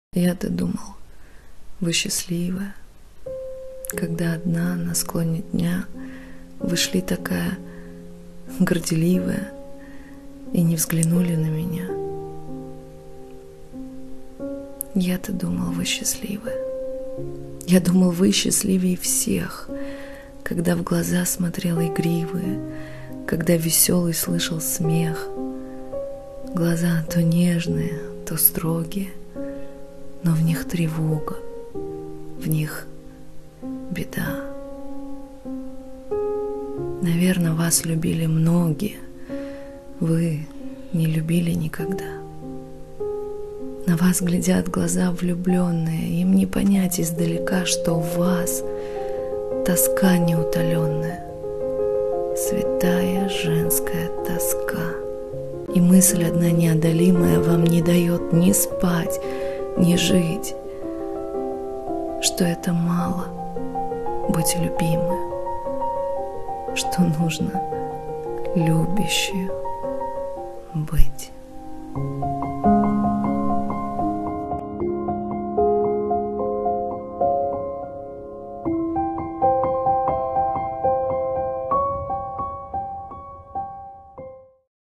А я-то думал, Вы счастливая | Автор стихотворения: Петр Градов
А я-то думал, Вы счастливая,
Когда одна на склоне дня
Вы шли такая горделивая
И не взглянули на меня.

Я думал, Вы счастливей всех,
Когда смотрел в глаза игривые,
Когда веселый слышал смех.

Глаза то нежные, то строгие,
Но в них тревога, в них беда.
Наверно, Вас любили многие.
Вы не любили никогда.

На Вас глядят глаза влюбленные.
Им не понять издалека,
Что в Вас тоска неутоленная,
Святая женская тоска.

И мысль одна неодолимая
Вам не дает ни спать, ни жить:
Что это мало – быть любимою,
Что надо любящею быть.

Святая, гордая, красивая…
Я слышу ваш веселый смех.

Автор стихотворения: Петр Градов